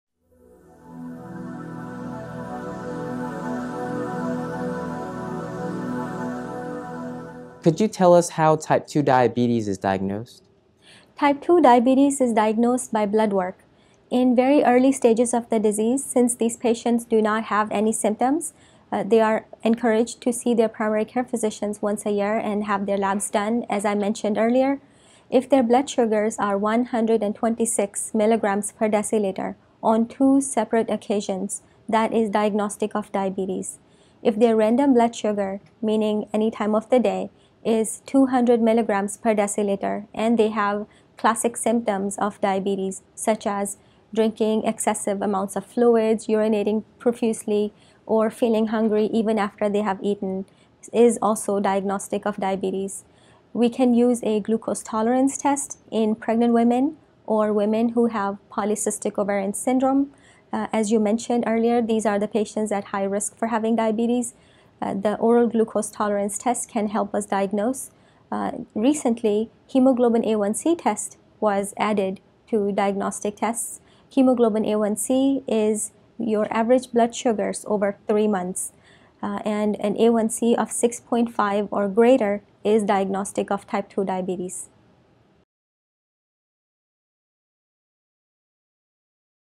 Diagnosis of type 2 diabetes
Diabetes experts describe the laboratory tests used to diagnose type 2 diabetes, and how these tests are interpreted. These tests include the fasting blood glucose test, oral glucose tolerance test and hemoglobin A1c test. It is important for timely diagnosis of type 2 diabetes as it has no symptoms in its early stages.